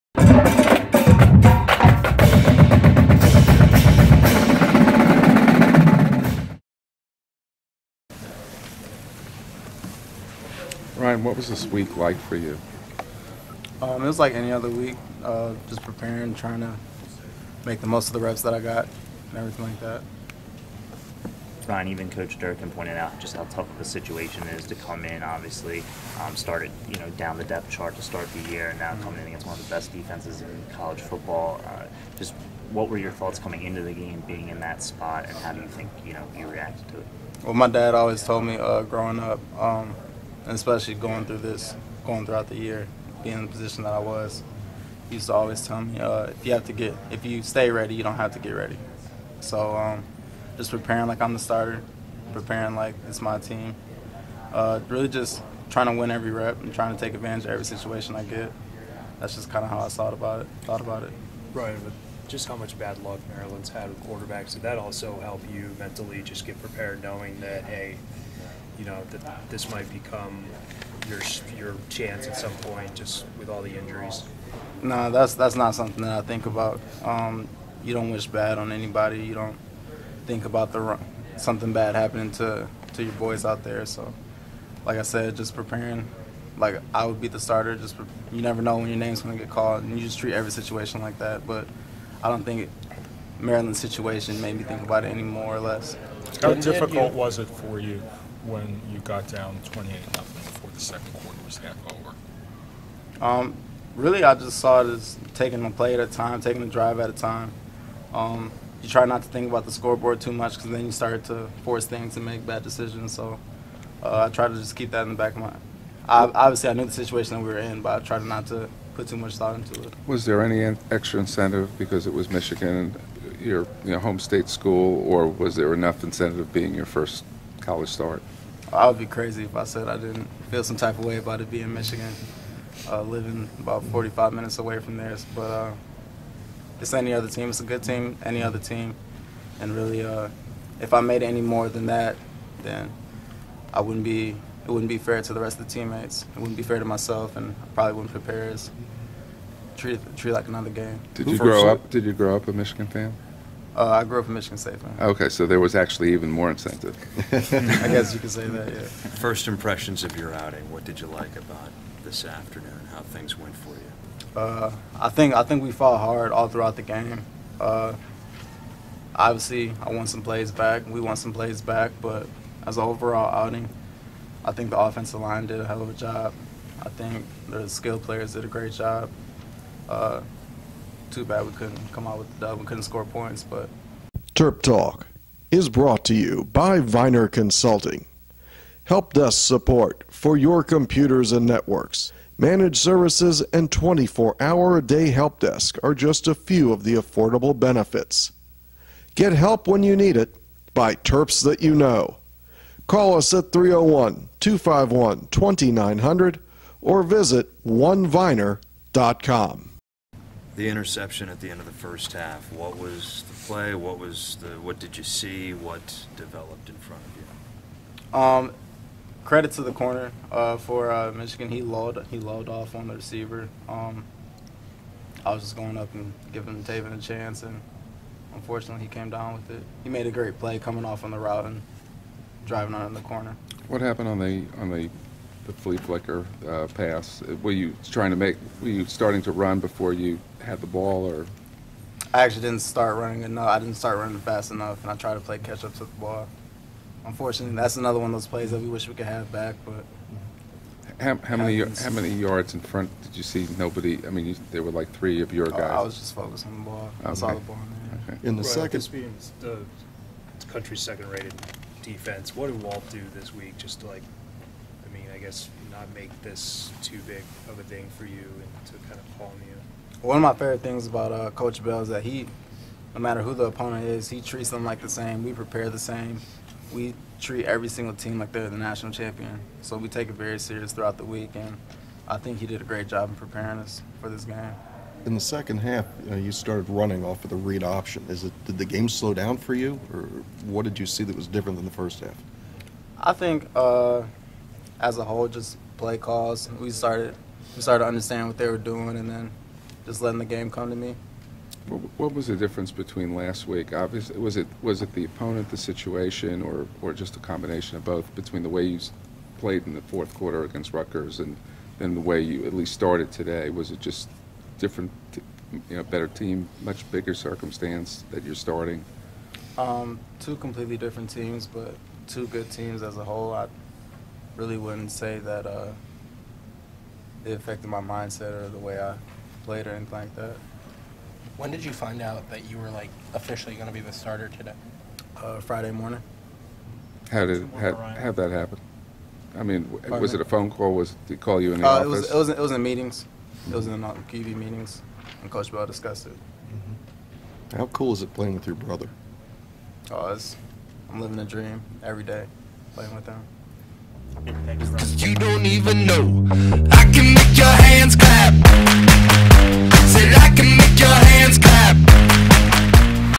Maryland Terps Football - QB Ryan Brand after Michigan game
Maryland's 4th starter at QB this season is Ryan Brand.  Here he is after the Michigan game.